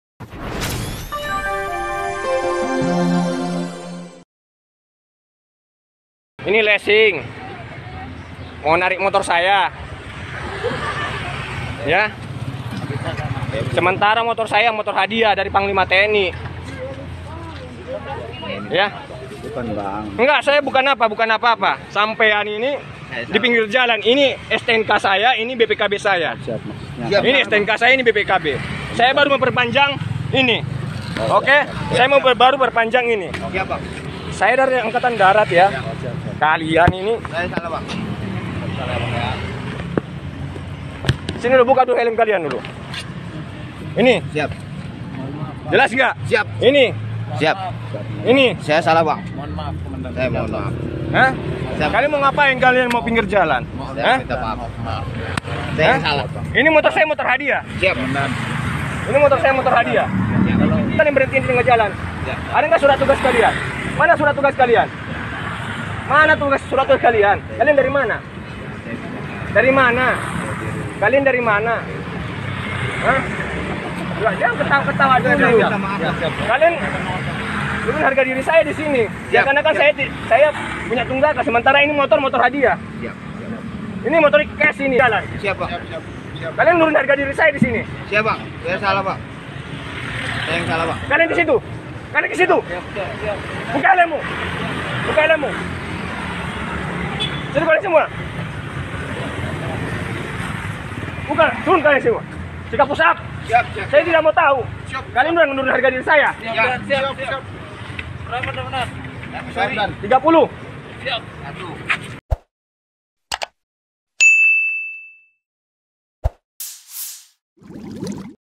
Hendak Tarik Motor TNI, Debt Collector Ini Kena Batunya
Sebuah video memperlihatkan sekelompok pria berprofesi sebagai leasing hendak menarik motor di jalan.

Pria yang diberhentikan merupakan anggota TNI.

Tak hanya itu, motor yang hendak ditarik ternyata hadiah dari Pangliam TNI.

Akibat tindakannya tersebut mereka dihukum dengan push up.

Video tersebut dibagikan oleh akun Instagram @ndorobeii, Kamis (22/10).